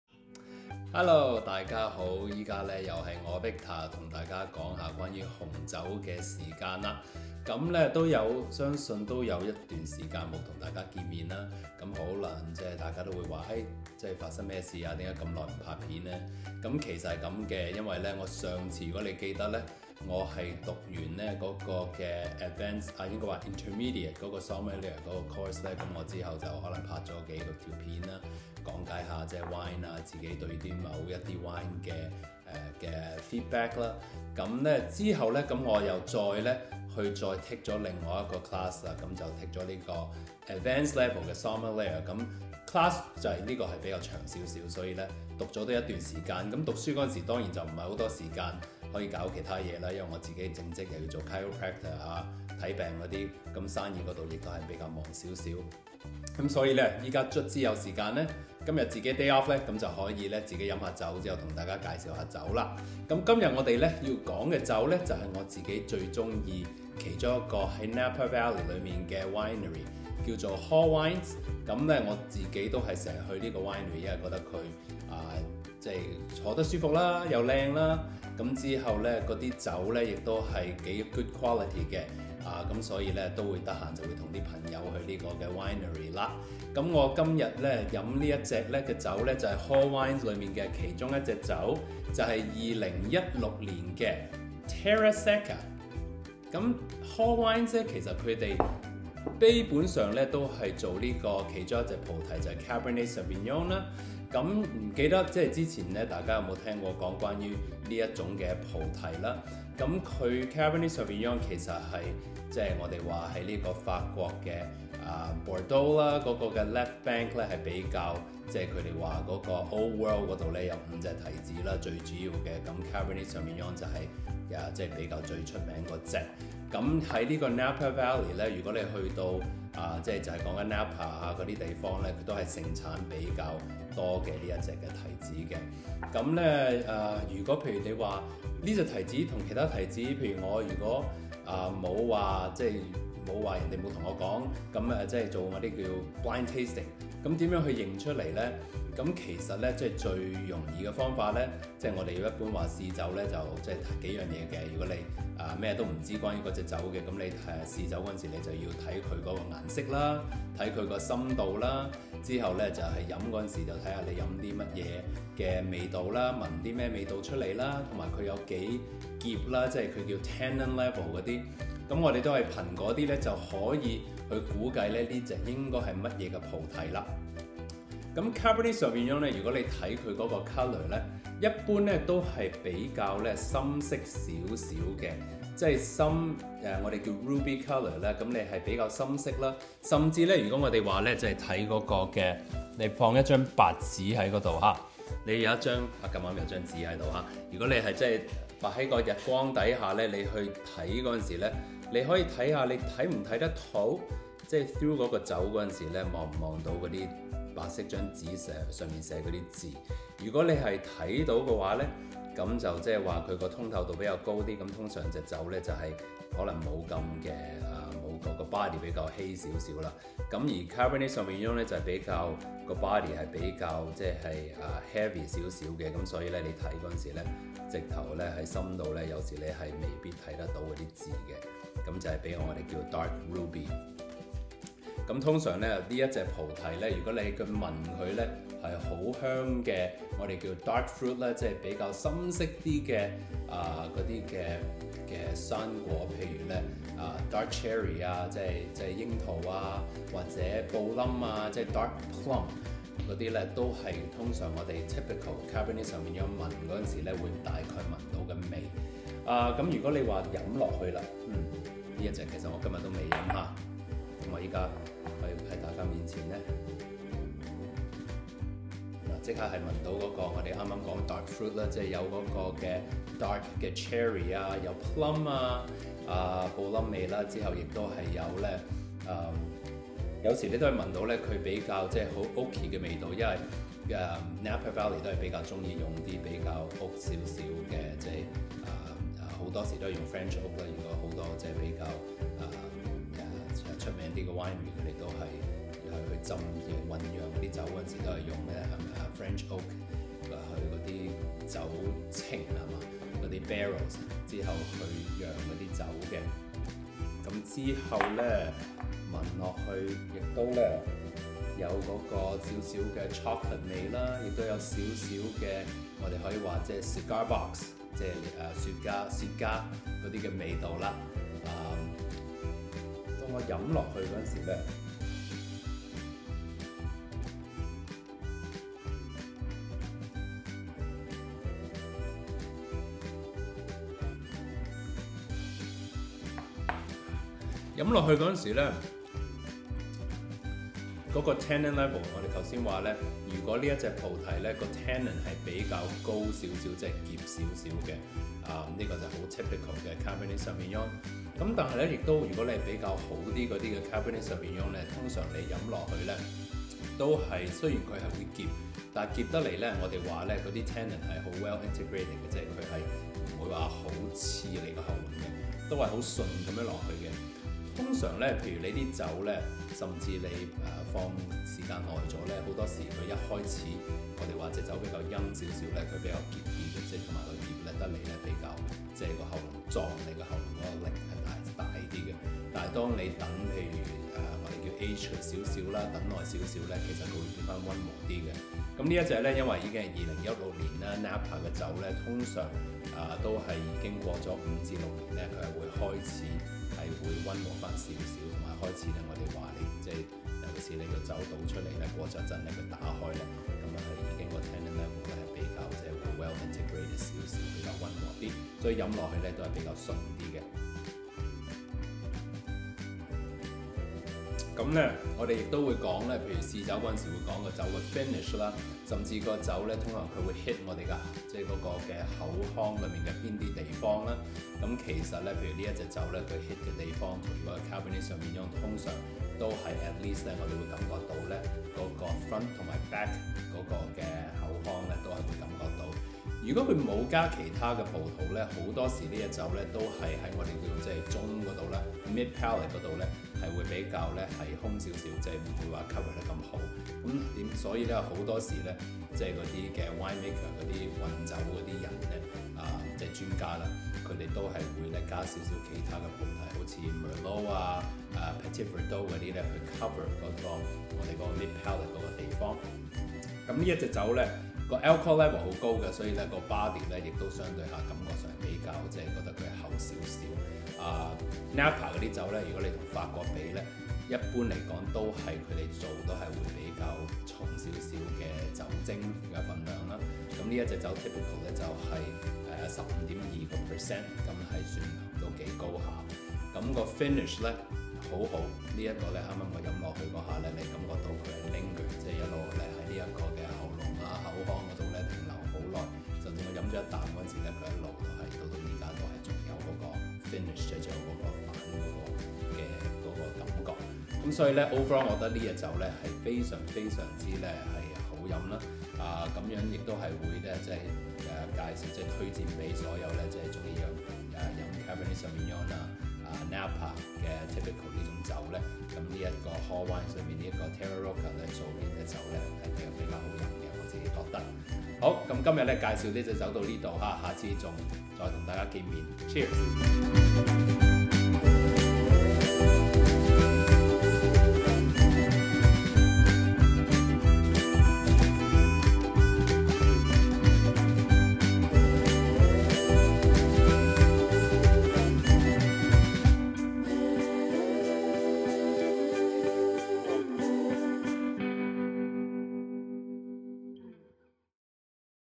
Victor 同大家介紹吓Hall Wines 嘅紅酒￼
Here I want to talk about one of the wines (Terra Secca 2016) by Hall Wines. 🍷